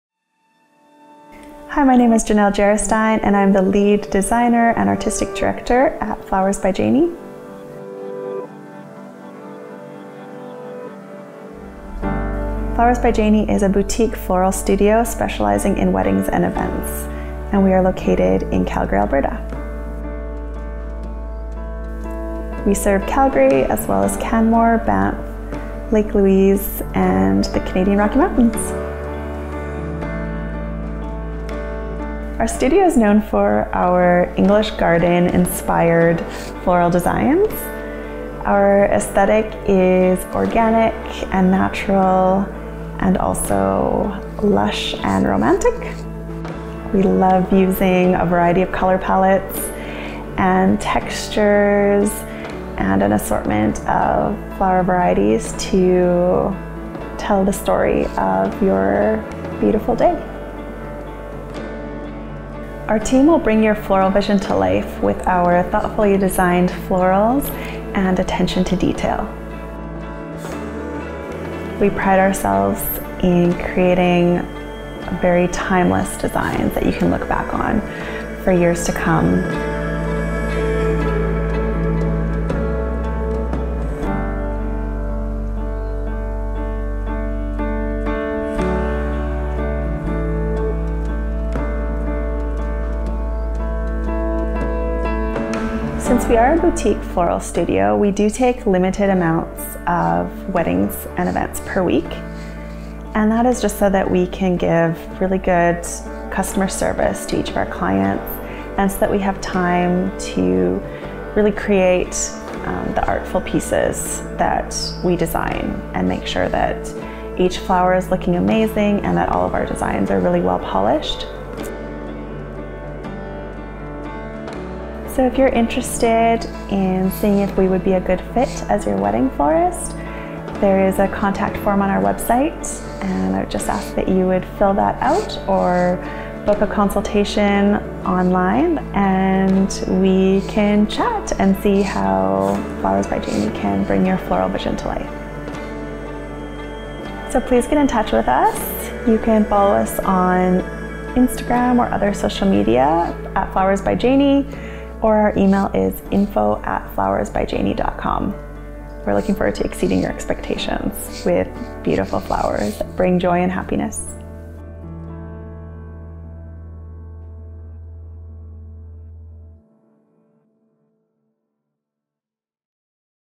Flowers by Janie About Us Video
Flowers by Janie is a boutique wedding floral studio located in Calgary Alberta. We serve Calgary, Canmore, Banff and the Canadian Rocky Mountains.

At Flowers by Janie, we are passionate about flowers and love creating artistic, quality floral designs that are natural, romantic and timeless with a lush English garden and European feel.

Watch the video to learn more about our floral studio, see some behind the scenes footage of us flowering and view our floral creations!